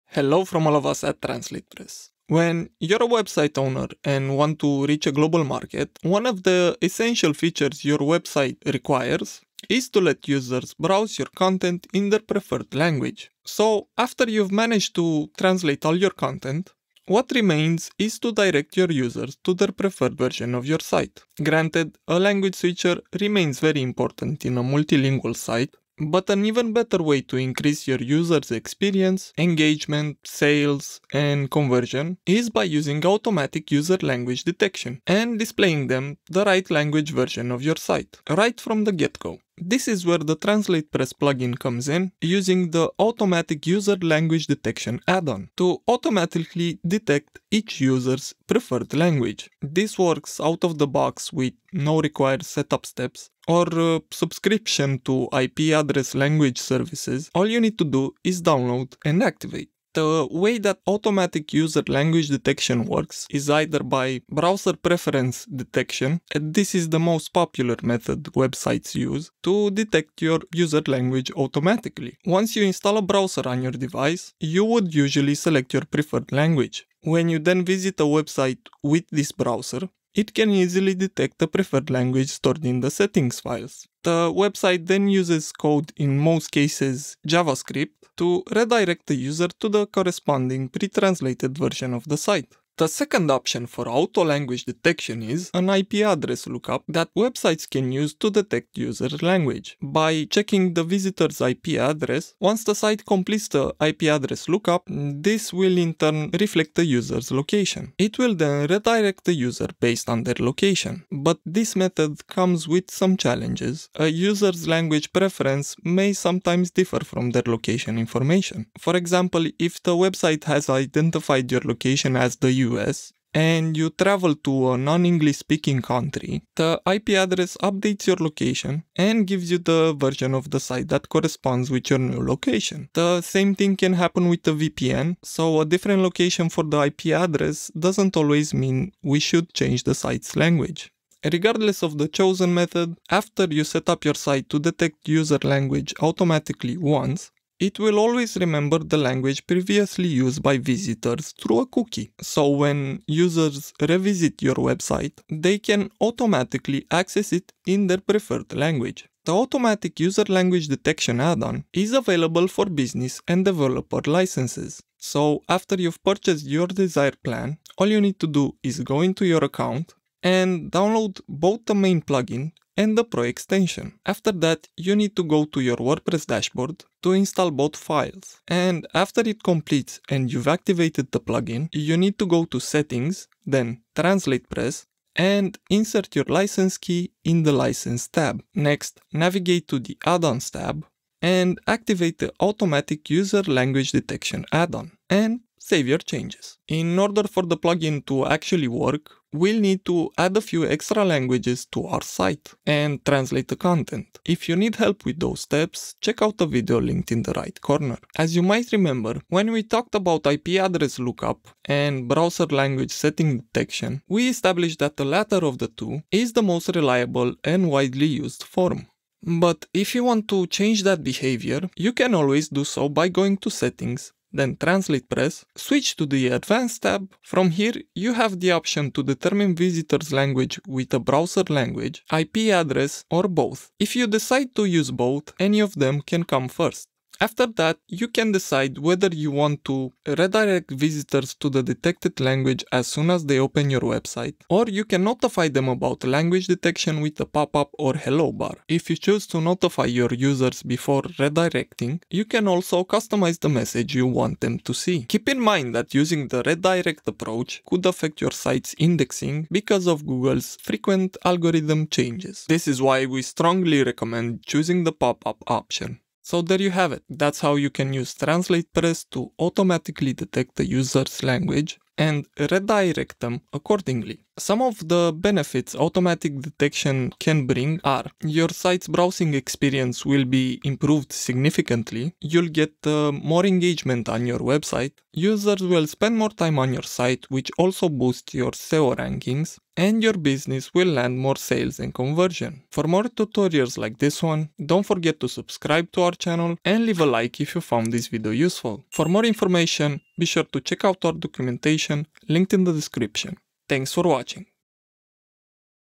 Auto Language Detection for Multilingual WordPress Sites (Automatically Detect User Language)
Learn how to automatically redirect your site visitors to their preferred language version of your website using automatic user language detection and the TranslatePress plugin.

TranslatePress is a freemium WordPress plugin that turns your website multilingual by letting you translate all of your website content using a simple visual translation interface. The plugin also comes with lots of add-ons for extended functionality, such as the Automatic User Language Detection add-on.

Chapters:
00:00 Intro
00:39 How Automatic User Language Detection Works
01:00 Browser Settings Language Detection
01:32 IP Address Language Detection
02:39 How to Automatically Detect User Language with TranslatePress
04:34 Why Redirect Users to Their Preferred Language?

Join our Facebook TranslatePress Community and get support from others just like you